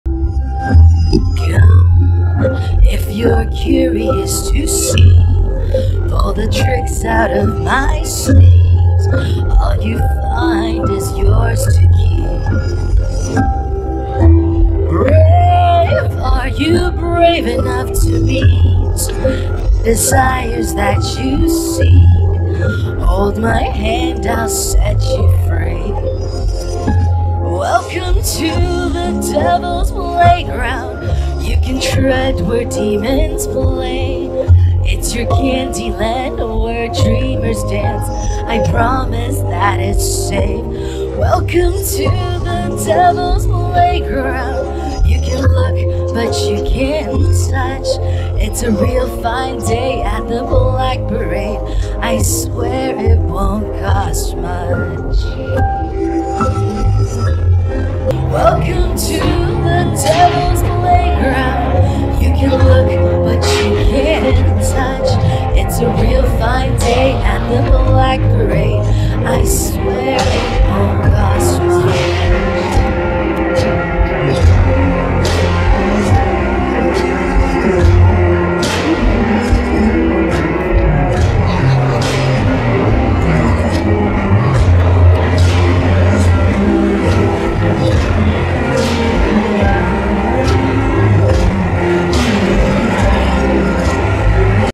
Teaser 01 Devils Playground Part One
Madame Rouge Presents the Most over-the-top Burlesque Show called The Devil's Playground. And now, we reveal the first of two parts. You have never experienced anything like this anywhere in the WORLD!!! Don't miss it! In Fact, watch it twice or three times to catch it all.